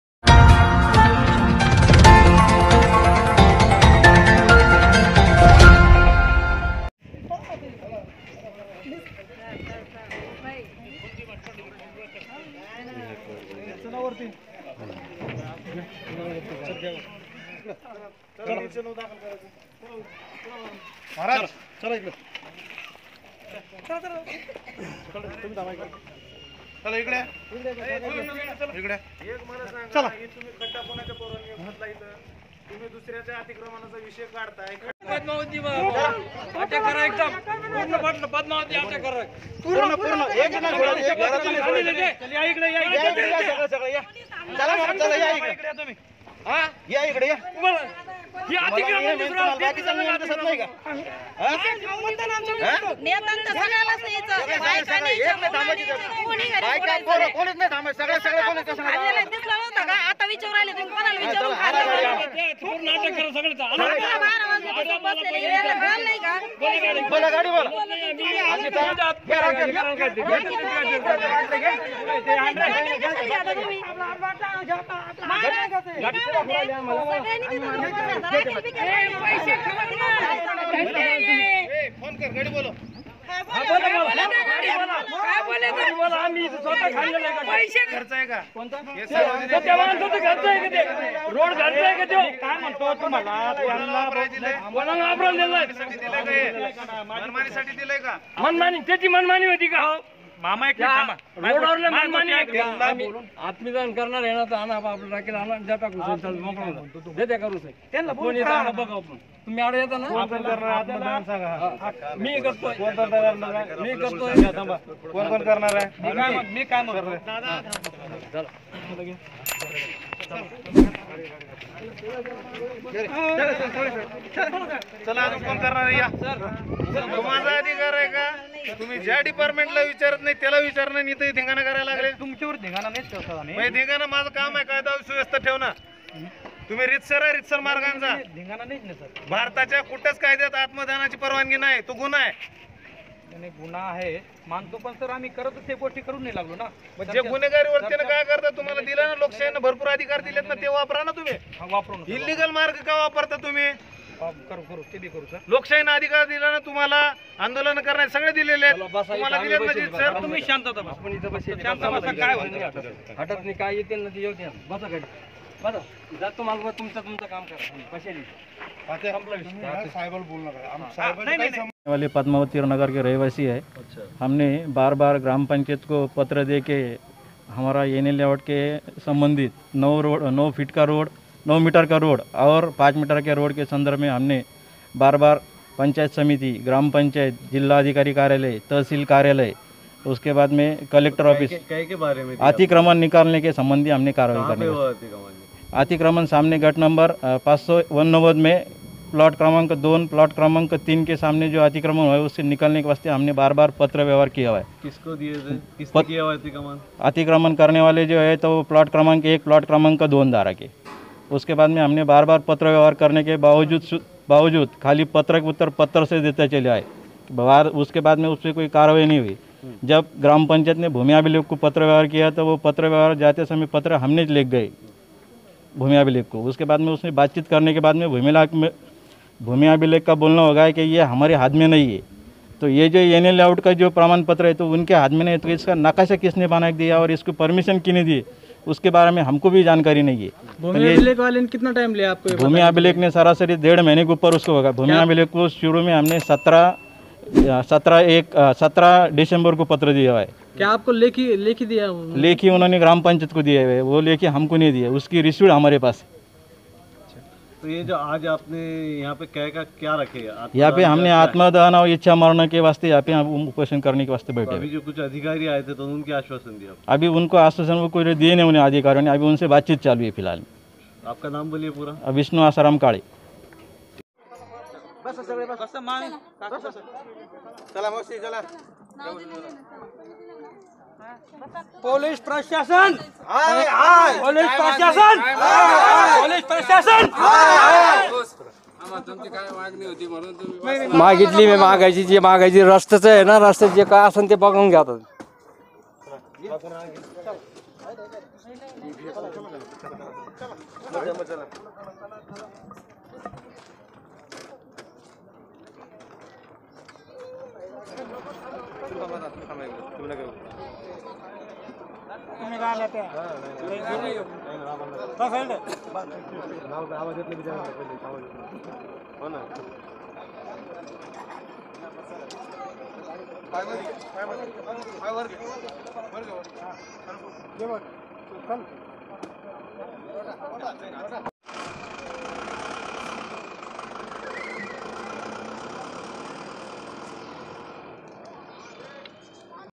Padmavati Colony Bidkin Me Atikarman Janta Aur Police Ke Bich Garma Garmi.
(Marathwada Voice)